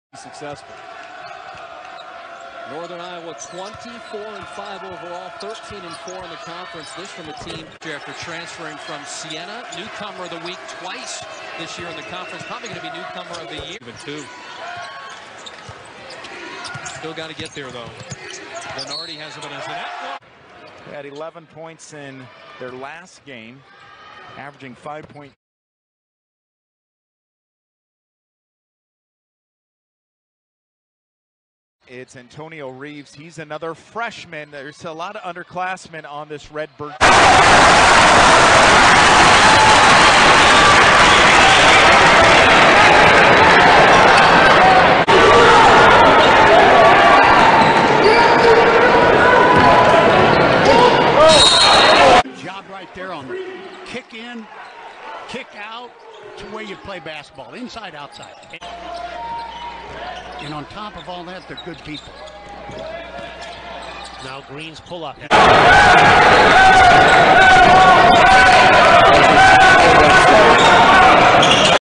AJ Green & Northern Iowa "Get" Action
In our program we call this a "Get" as in pass it and go get it...clever I know. Good option to play a 2 man game and force the defense into a tough position with how to guard it. Also, AJ Green of Northern Iowa is vastly underrated!